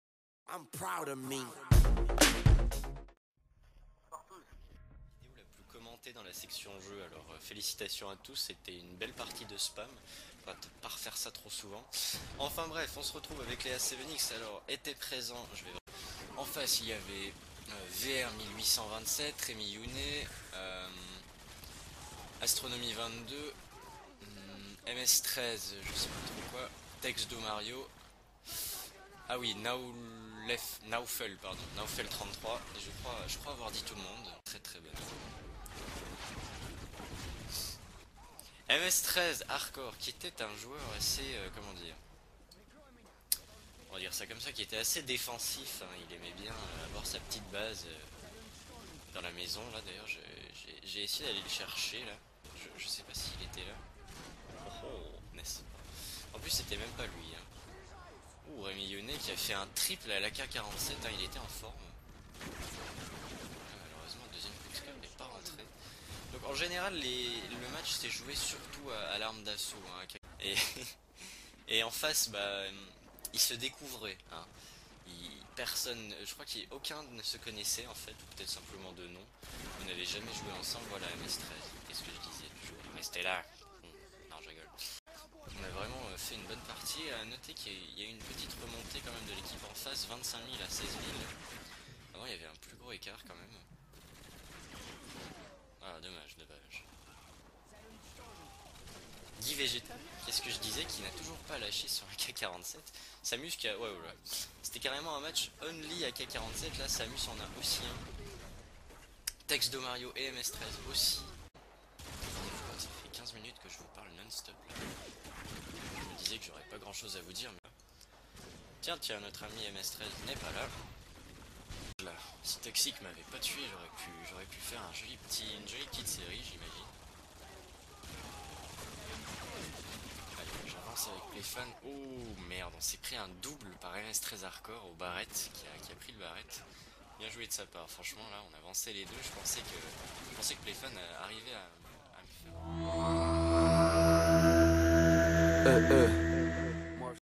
► MS13-HaRKoR : A7X Vs Unknow . Tué Diablox9 çà n'a pas de prix ◄
Match de A7X Contre les Unknow ( inconnus ) Match organisé par Diablox9 . Sa team vs des inconnus tirés au hazard . Score final 54 000 à 35 200 . Match remporté par les A7X . Très beau match ! Vidéo en entière à regarder sur la page youtube de Diablox9